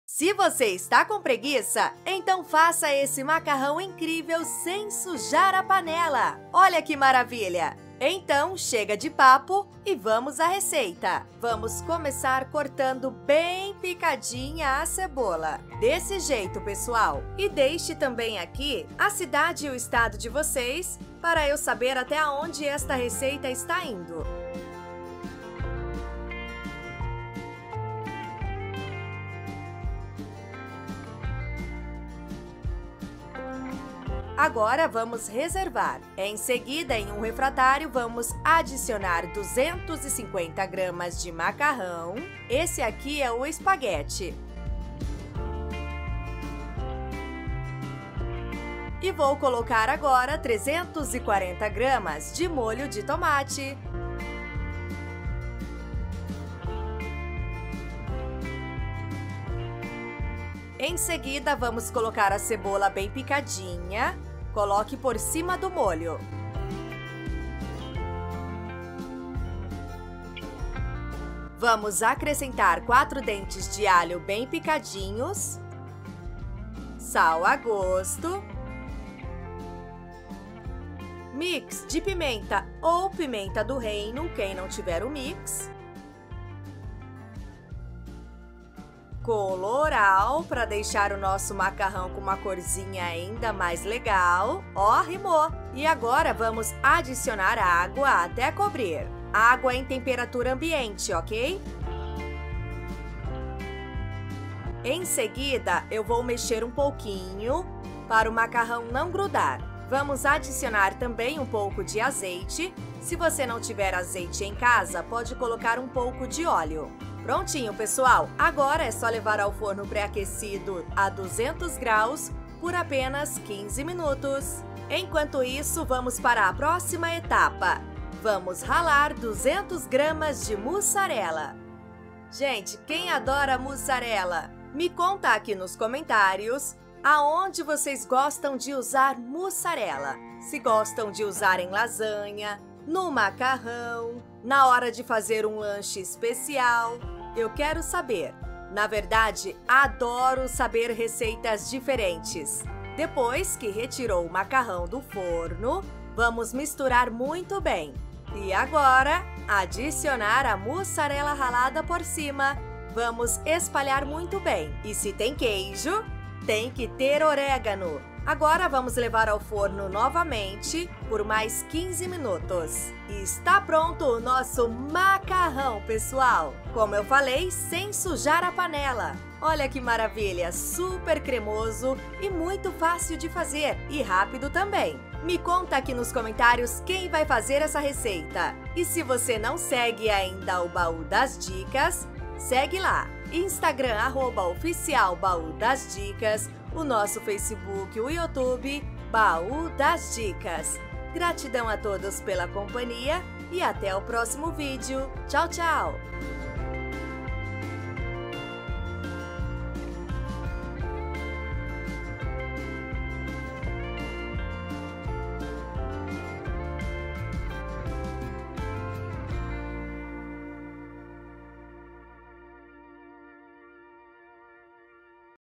Macarrão de Preguiçoso, sem sujar panela!
Ingredientes

-Cebola
-Macarrão ( 250g )
-Molho de Tomate ( 340g )
-Alho ( 4 dentes )
-Sal
-Mix de Pimenta
-Colorau
-Água
-Azeite
-Muçarela ( 200g )
-Orégano

Ja conferiu os ingredientes? Deixa um "Amei" aqui para que esse comentario fique no topo e que TODOS Vejam, rs